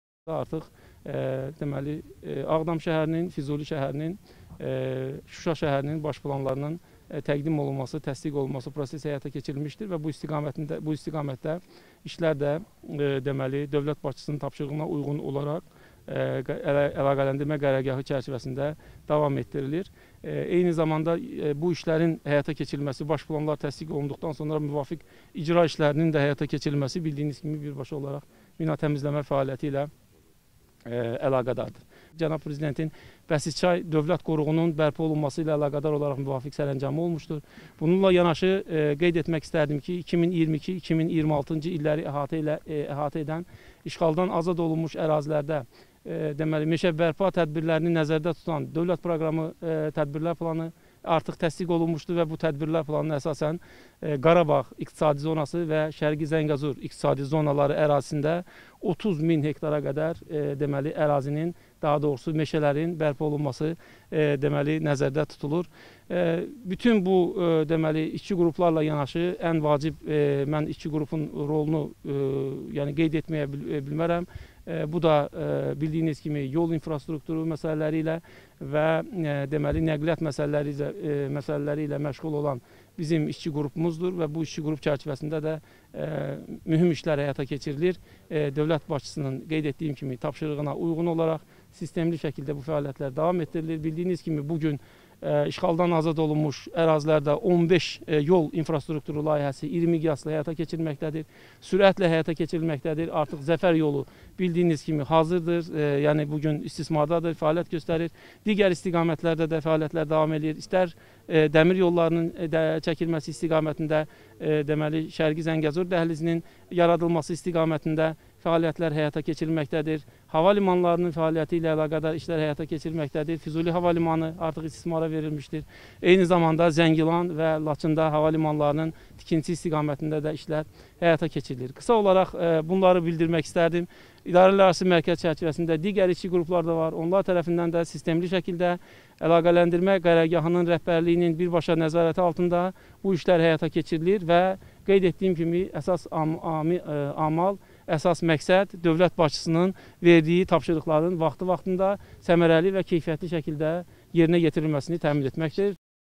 Qarabağ və Şərqi Zəngəzur iqtisadi zonalarında 30 min hektaradək meşə bərpa olunacaq
Ağdam, Füzuli və Şuşa şəhərlərinin baş planlarının hazırlanaraq təsdiq olunması prosesi həyata keçirilib. Baş planlara əsasən müvafiq işlərin görülməsi birbaşa minatəmizləmə fəaliyyəti ilə əlaqədardır.
AZƏRTAC-ın bölgə müxbiri xəbər verir ki, bu sözləri Ağdamda ANAMA-nın mobil sahə düşərgəsinin açılışında iştirak edən Prezident Administrasiyasının Xarici siyasət məsələləri şöbəsinin müdir müavini, Əlaqələndirmə Qərargahının İdarələrarası mərkəzi katibliyinin rəhbəri Nüsrət Süleymanov söyləyib.